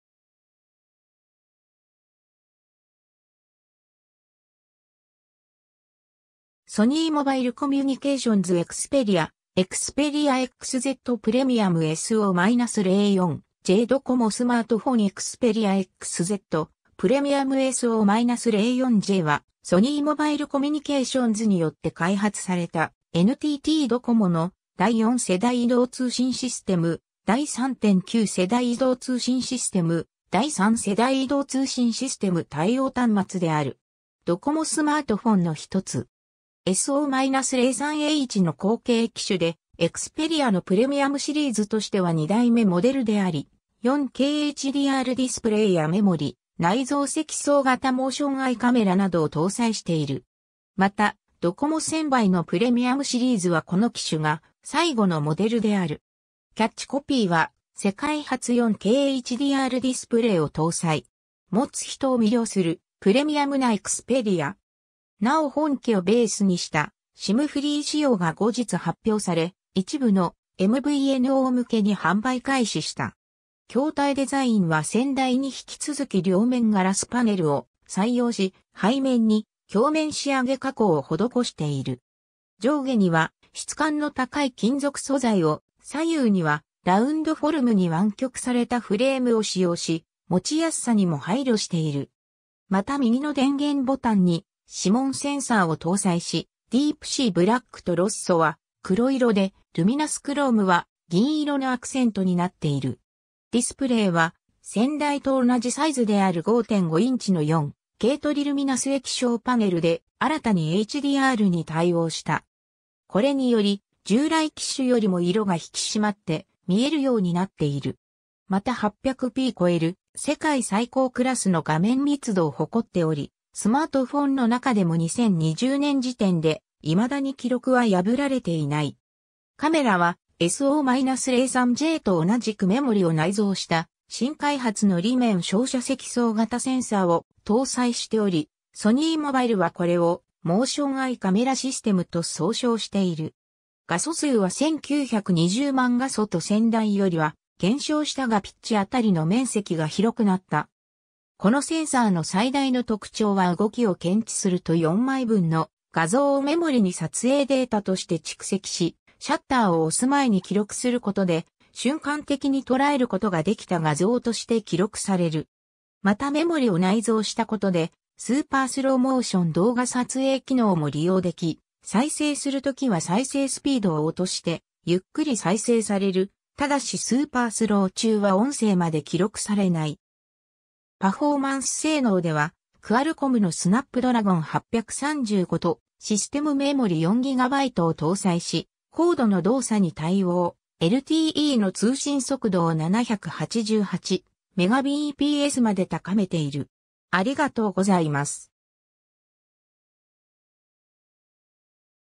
SO-04J
携帯電話端末_(NTTドコモ_第4世代)
携帯電話端末_(NTTドコモ_第3.9世代)
携帯電話端末_(ソニーモバイルコミュニケーションズ)
Android端末_(ソニー)
ソニーモバイルコミュニケーションズ 」 Xperia 」 Xperia XZ Premium 」 SO-04J ドコモ スマートフォン Xperia XZ Premium SO-04J（ドコモ スマートフォン エクスペリア エックスゼット プレミアム エスオーゼロヨンジェイ）は、ソニーモバイルコミュニケーションズによって開発された、NTTドコモの第4世代移動通信システム（PREMIUM 4G）・第3.9世代移動通信システム（Xi）・第3世代移動通信システム（FOMA）対応端末である。
ドコモ スマートフォン（第2期）のひとつ。
 SO-03Hの後継機種で、XperiaのPremiumシリーズとしては二代目モデルであり4K HDRディスプレイやメモリ内蔵積層型Motion Eyeカメラなどを搭載している。
またドコモ専売のPremiumシリーズはこの機種が最後のモデルである。
キャッチコピーは「世界初 4K HDRディスプレイを搭載。持つ人を魅了する、プレミアムなXperia。」。
なお本機をベースにしたSIMフリー仕様が後日発表され、一部のMVNO向けに販売開始した。
 筐体デザインは先代に引き続き両面ガラスパネルを採用し、背面に鏡面仕上げ加工を施している。
上下には質感の高い金属素材を、左右にはラウンドフォルムに湾曲されたフレームを使用し持ちやすさにも配慮している。
また右の電源ボタンに指紋センサーを搭載し、Deepsea BlackとRossoは黒色でLuminous Chromeは銀色のアクセントになっている。
 ディスプレイは先代と同じサイズである5.5インチの4Kトリルミナス液晶パネルで、新たにHDRに対応した。
これにより従来機種よりも色が引き締まって見えるようになっている。
また800ppi超える世界最高クラスの画面密度を誇っており、スマートフォンの中でも2020年時点で未だに記録は破られていない。
 カメラはSO-03Jと同じくメモリ(DRAM)を内蔵した新開発の裏面照射積層型センサーを搭載しており、ソニーモバイルはこれを「Motion Eyeカメラシステム」と総称している。
画素数は1920万画素と先代よりは減少したがピッチ辺りの面積が広くなった。
このセンサーの最大の特徴は動きを検知すると4枚分の画像をメモリに撮影データとして蓄積し、シャッターを押す前に記録することで瞬間的に捉えることができた画像として記録される。
またメモリを内蔵したことでスーパースローモーション動画撮影機能も利用でき、再生するときは再生スピードを落としてゆっくり再生される(但しスーパースロー中は音声まで記録されない。
 パフォーマンス性能では QualcommのSnapdragon 835とシステムメモリ4GBを搭載し、高度の動作に対応、LTEの通信速度を788Mbpsまで高めている。